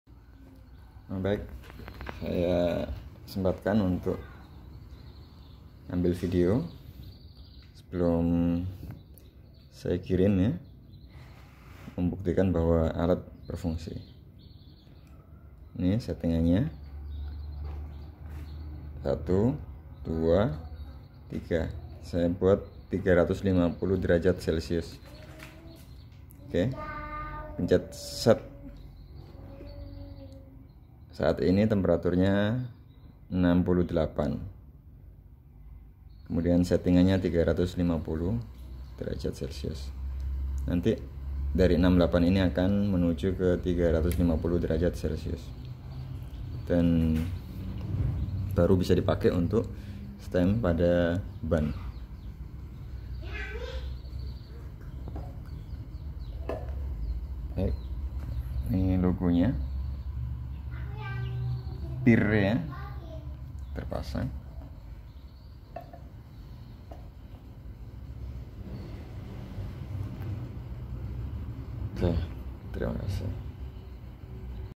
Hot stamp dengan logo custom PIR
power: 600 watt
daya: 220 V
Temperature controller: 0-400°C

alat ini berfungsi sebagai brander pada objek dengan logo yang akan membentuk efek emboss.